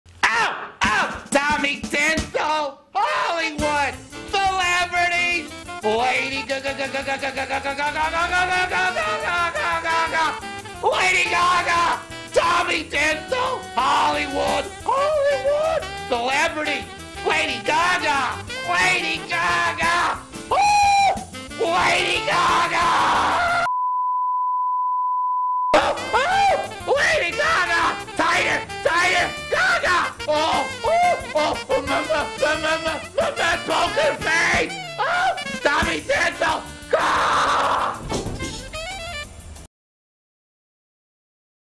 Lady Gaga Hollywood gossip
Tommy Tinsel effuses all on Lady Gaga. The things he says... Oh Oh Oh.